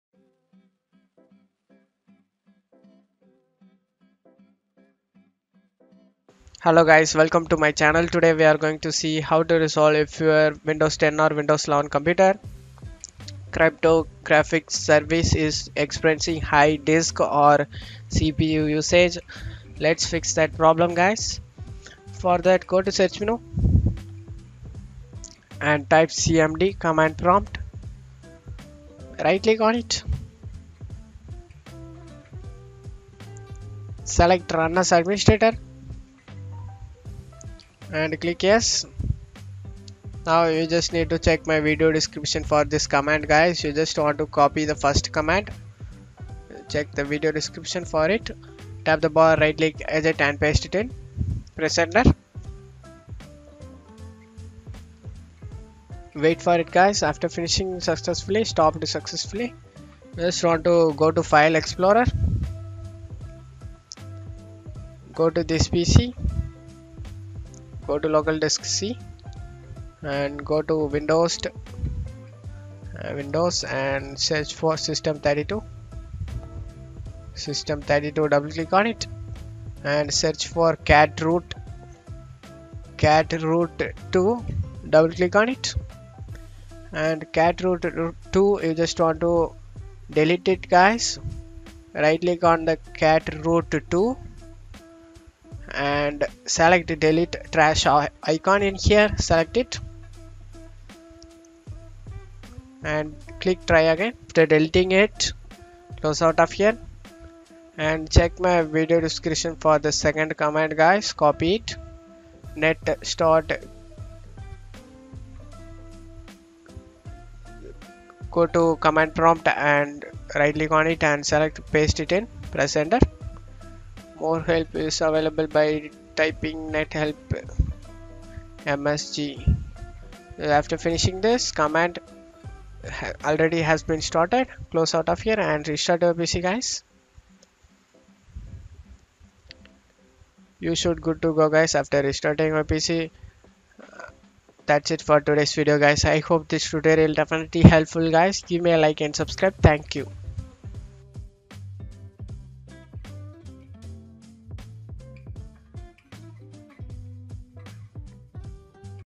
How To Fix Cryptographic Services High Disk Usage or CPU Usage in Windows 10/11
Issues addressed in this tutorial:
service host cryptographic services high disk usage windows 10